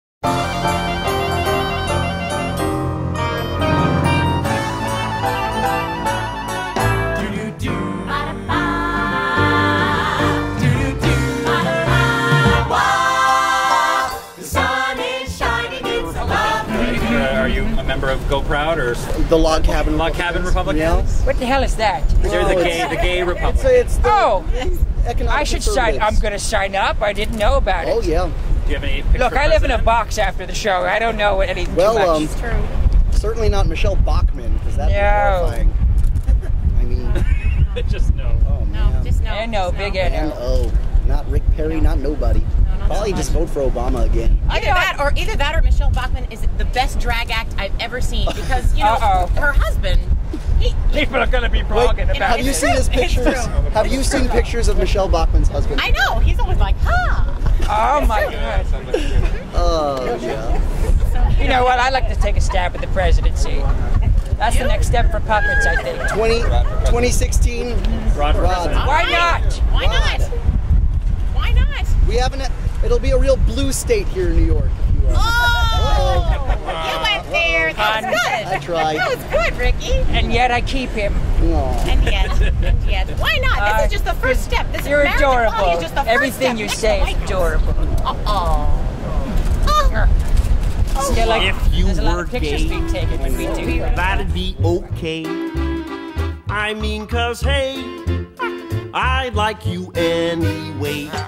Avenue Q on Michele Bachmann
On July 24, 2011, New York began issuing legal marriage licenses to lesbian and gay couples. Among those couples celebrating their union were
Avenue Q's Rod and Ricky.

The following video reveals some of Rod, Ricky and Kate Monster's thoughts on Gay Republicans, Puppets running for President...and Michele Bachmann.

Please note: the views expressed in this video do not necessarily reflect those of the producers or creative staff of Avenue Q.

(but they might)